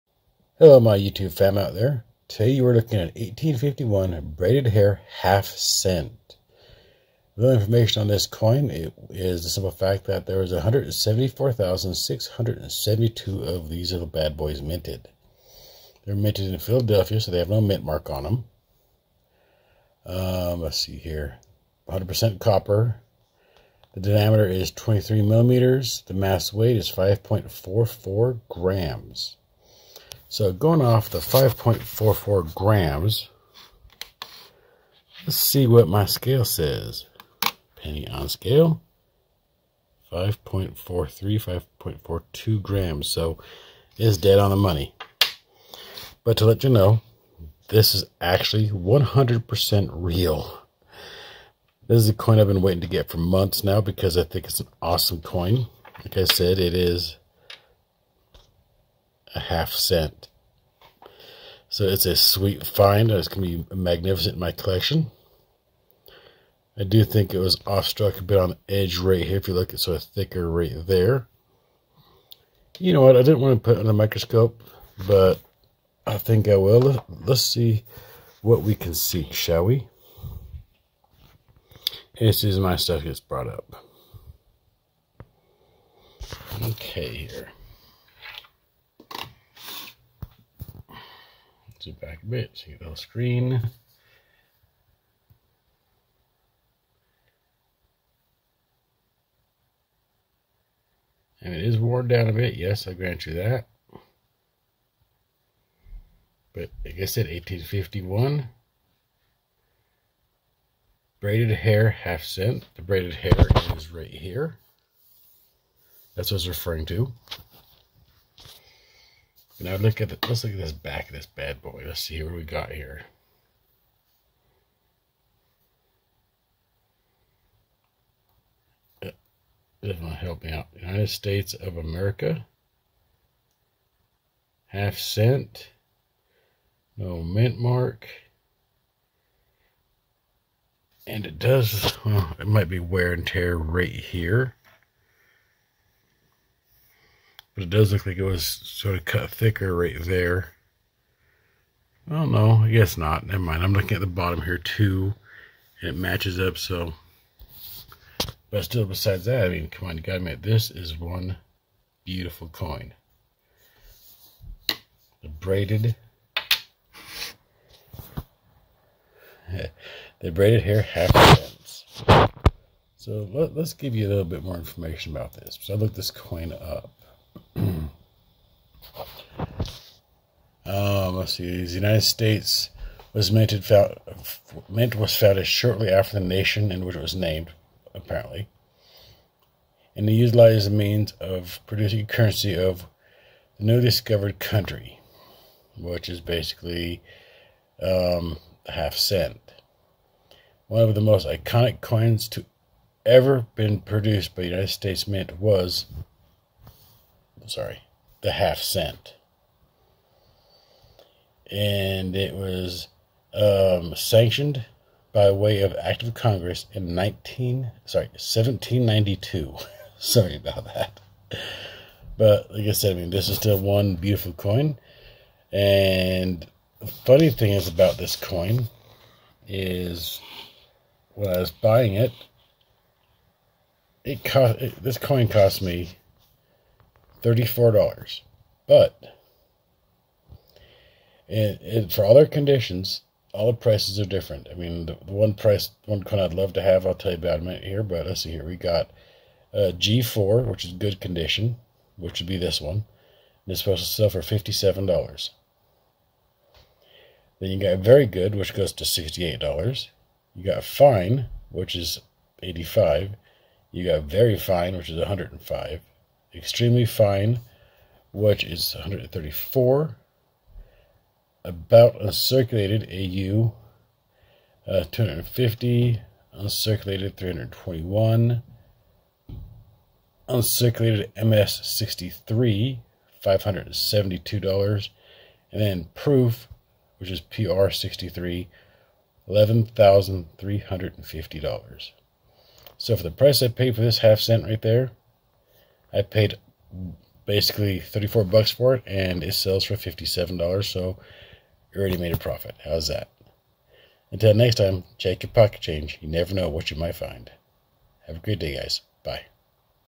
Whats in your pocket ( The Braided Hair Half Cent )
Here we look at the most iconic coin minted in the United States.
The Braided Hair 1/2 cent